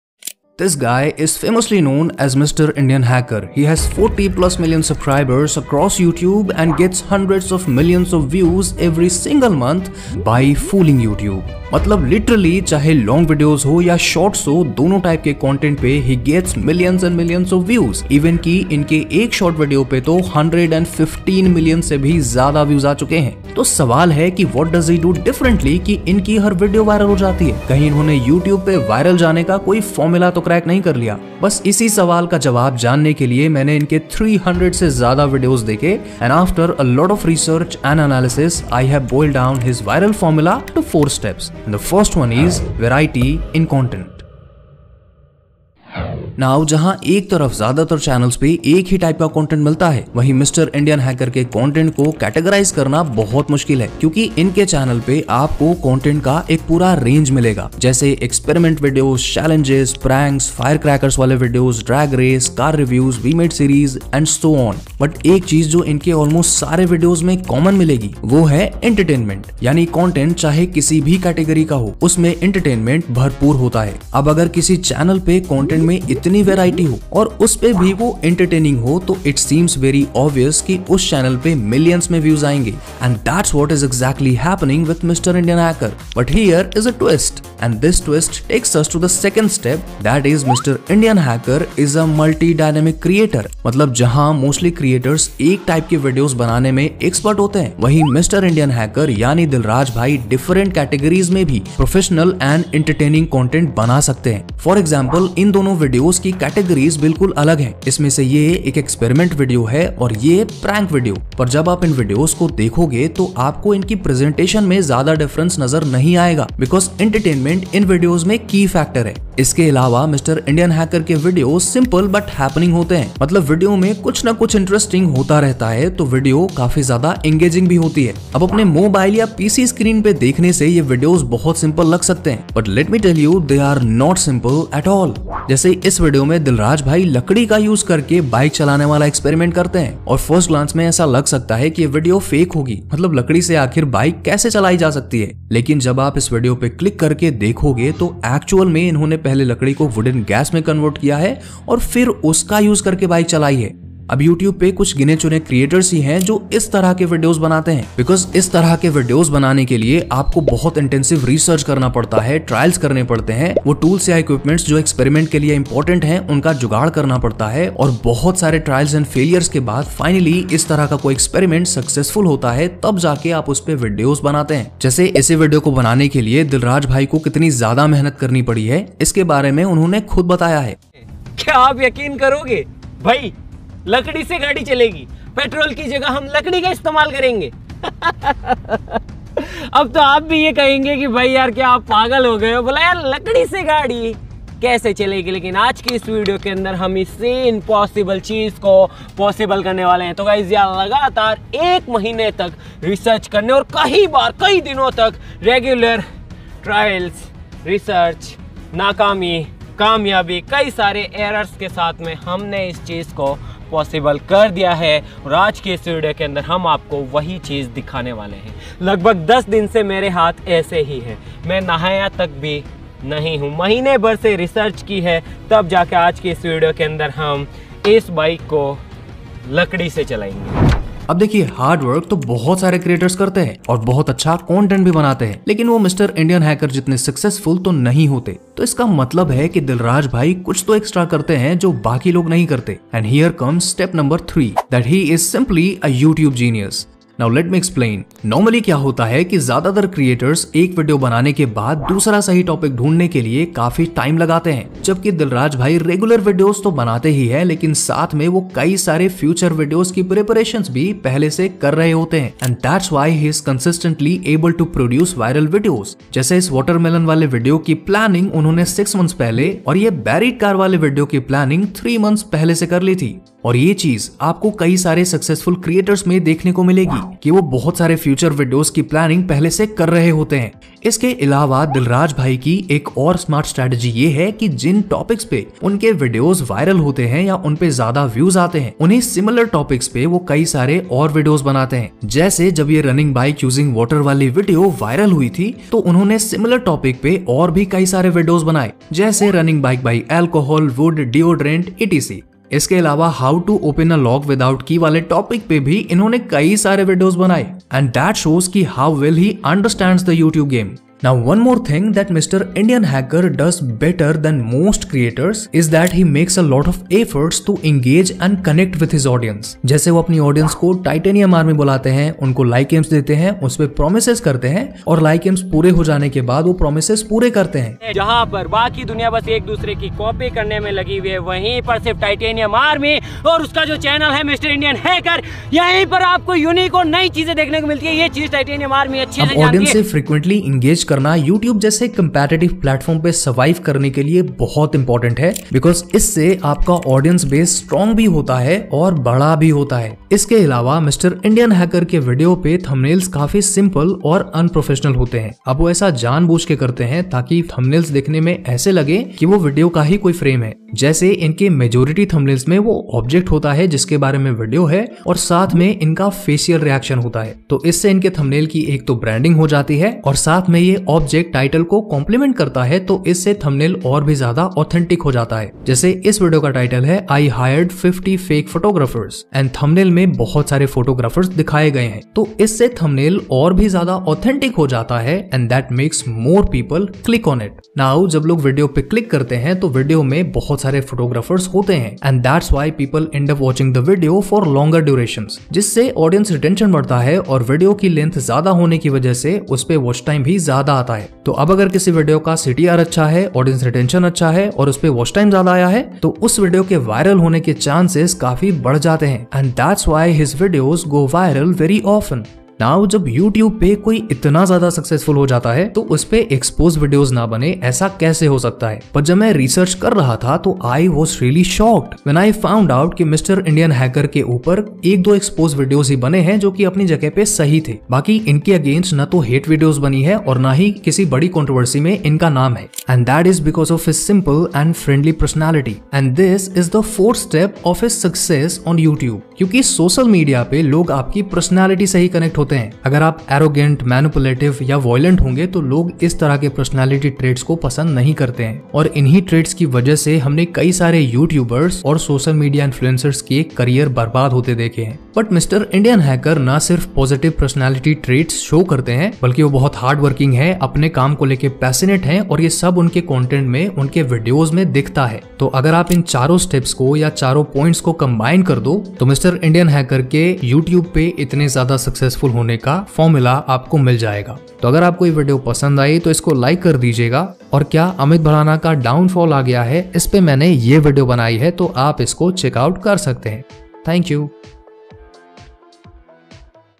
is NOT a YouTube Genius (Here's why?) 😒
MR. INDIAN HACKER ने Mustang से गाय का चारा ढोया, Viral हुई Youtuber

○ Microphone (Maono au-a04)
○ Microphone (Boya BYM1)
○ Backdrop (Background)
○ Voice filter (Foam)
○ Earphones Recommended
○ Laptop (Recommended)